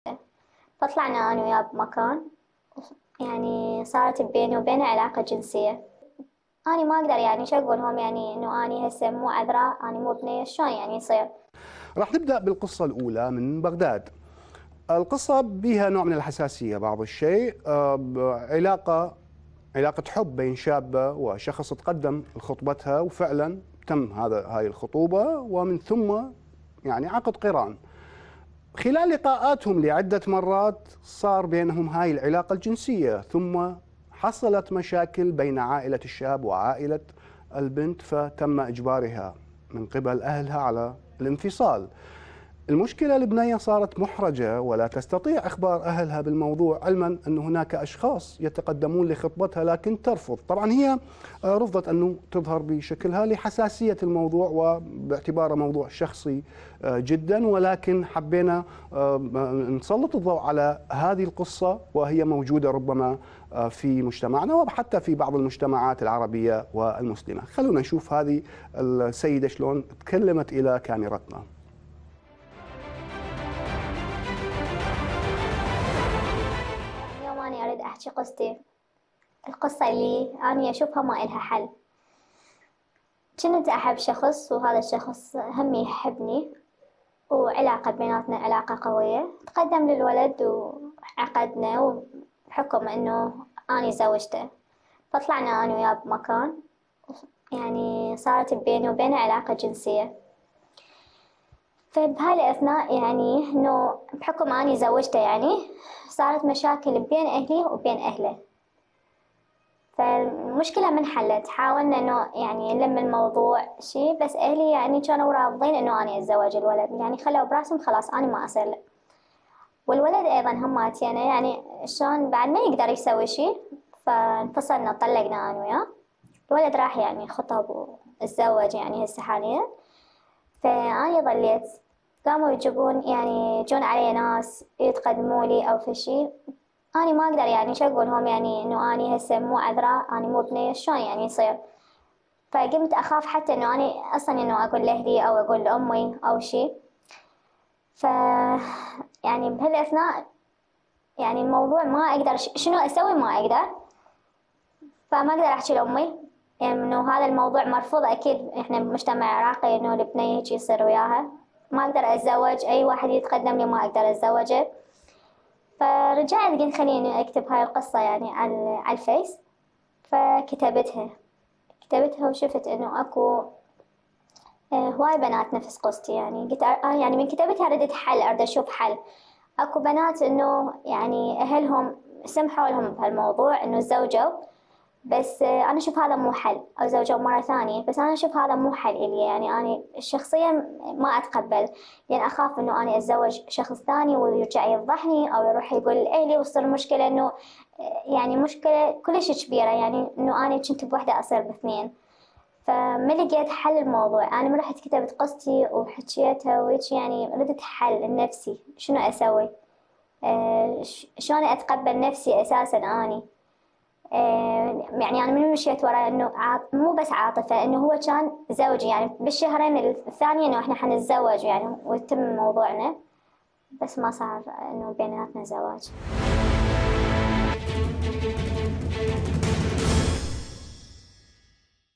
عراقية تروي تفاصيلا صادمة حول الدخول قبل الزواج
دخل بها قبل حفل الزفاف .. فحدثت المصيبة
عراقية تروي تفاصيل صادمة حول الدخول ما قبل الزواج
موقع قناة التغيير الفضائية  الالكتروني